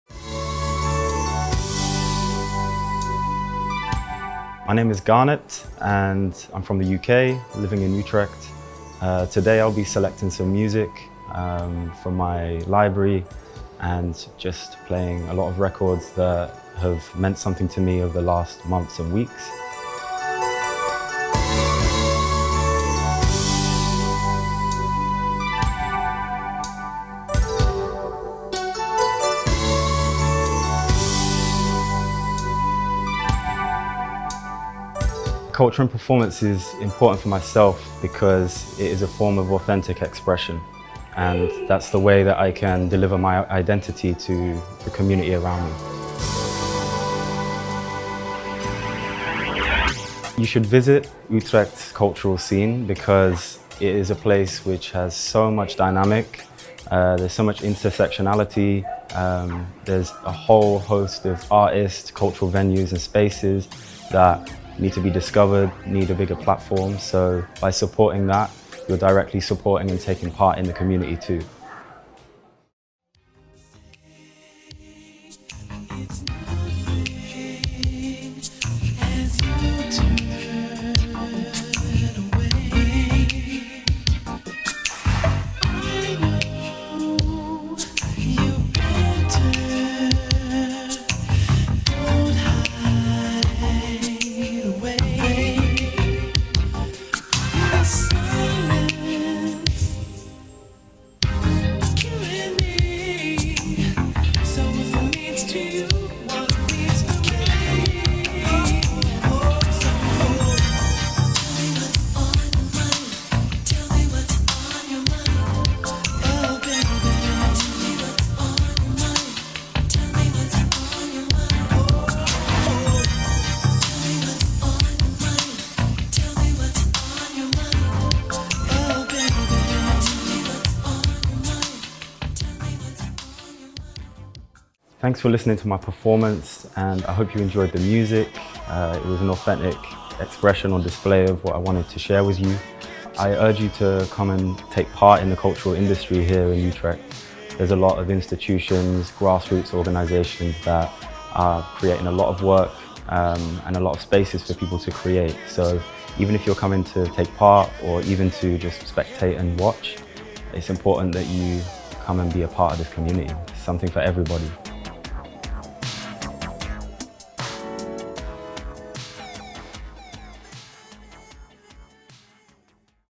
UITalage Utrecht - Garnett Stranded FM
In deze video: Garnett Stranded FM
Deze serie aan video's is gemaakt door Thomas Endeman.

Stranded FM is een online radiostation gevestigd in Utrecht. In de studio streamen residents en gast DJ's radioshows die variëren van podcasts tot mixes, live te beluisteren Garnett is met zijn twee uur durende show 'Soon Come' iedere twee maanden te beluisteren op Stranded FM. Daarnaast heeft hij de afgelopen tijd veel optredens gedaan voor o.a. de utrechtse club WAS., Lovelee Radio, Radio Tempo Não Para en RRFM.